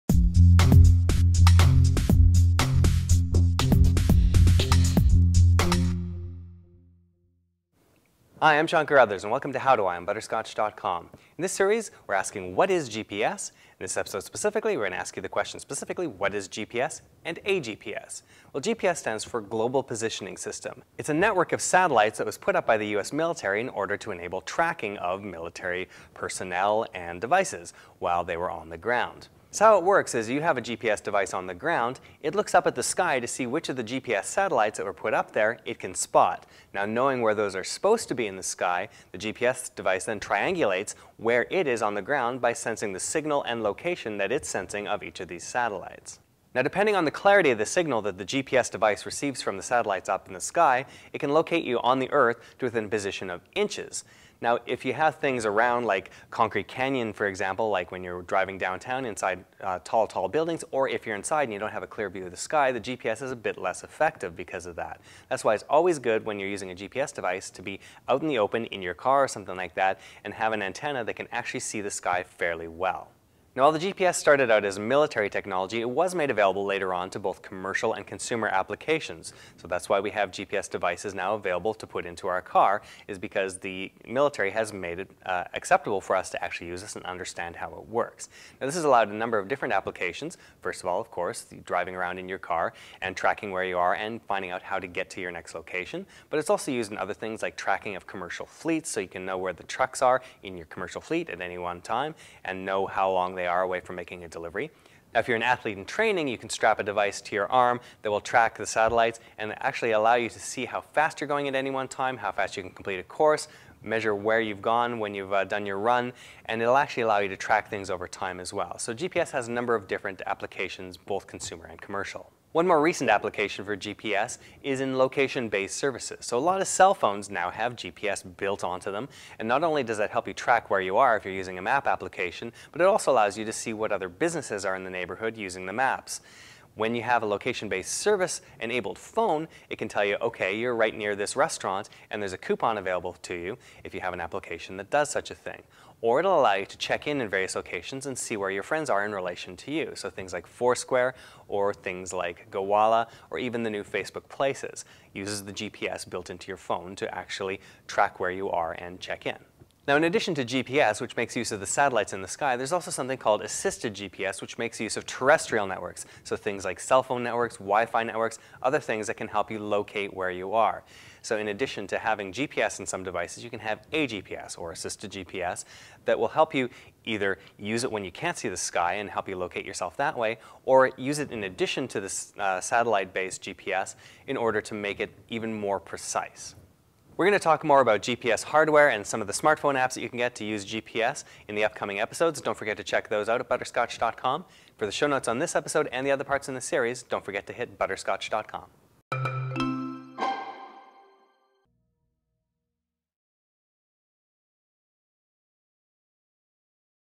What do GPS and AGPS mean
Global Positioning System (GPS) and was developed by the US military for the purpose of satellite navigation and the tracking of military units. It has evolved into a way of determining locations and plotting maps that's now universally used by everyone @ butterscotch.com